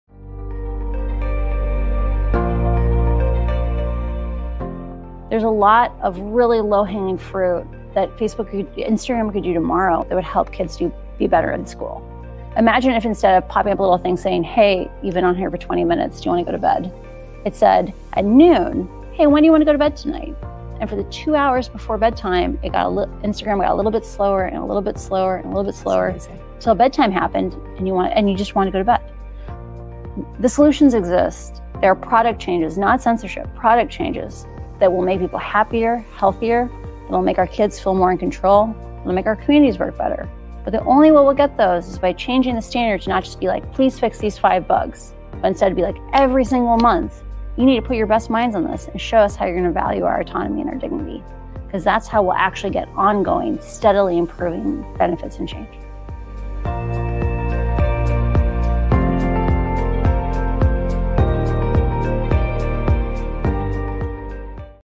Frances Haugen Talks About Accountability and Transparency on Social Media
What's happening to our kids? Listen to this clip of Frances Haugen, an advocate for accountability & transparency in social media, during her discussion with the American Federation of Teacher's Randi Weingarten about how we need more accountability for social media companies.